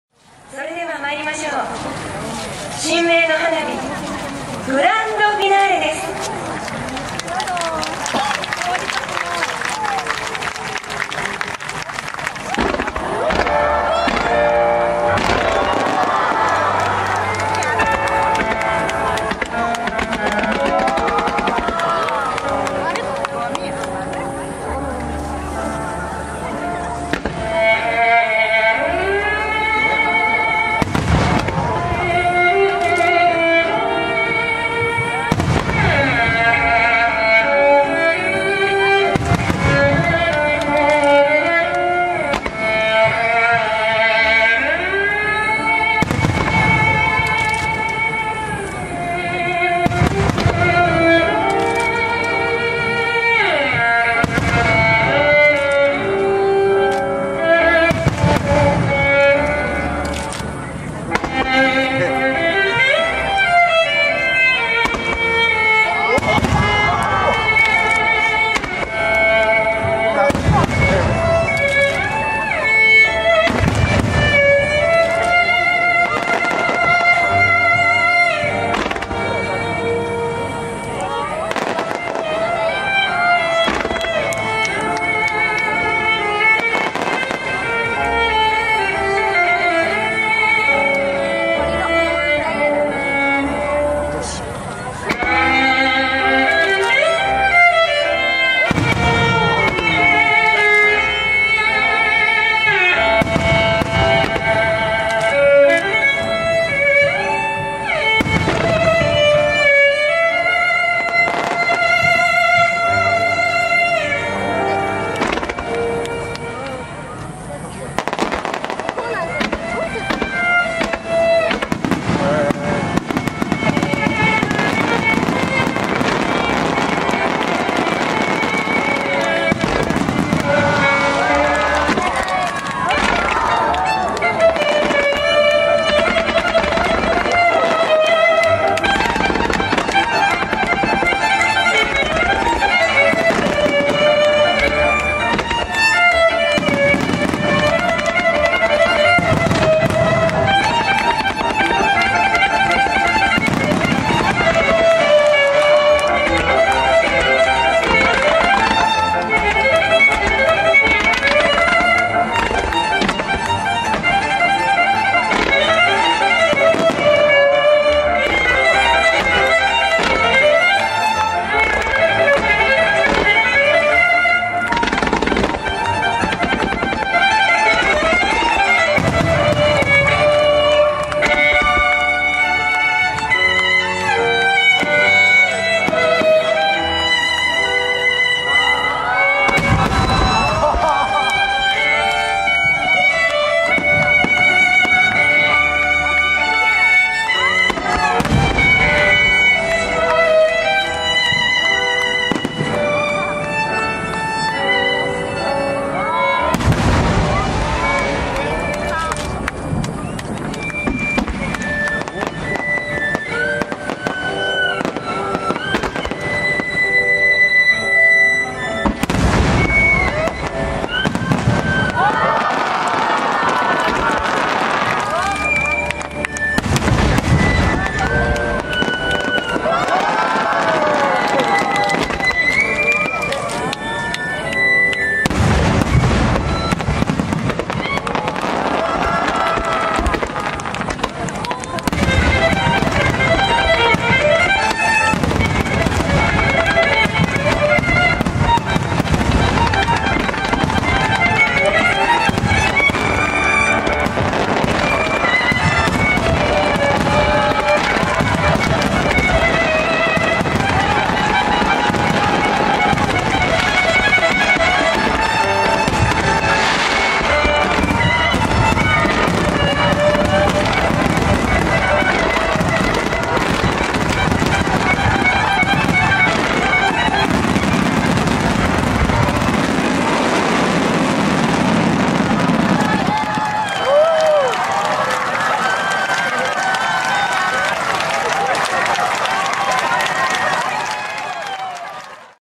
【魚眼全景】20150807神明の花火グランドフィナーレ
神明の花火ありがとう！そして新規投入のivis miniはいい仕事をしてくれました。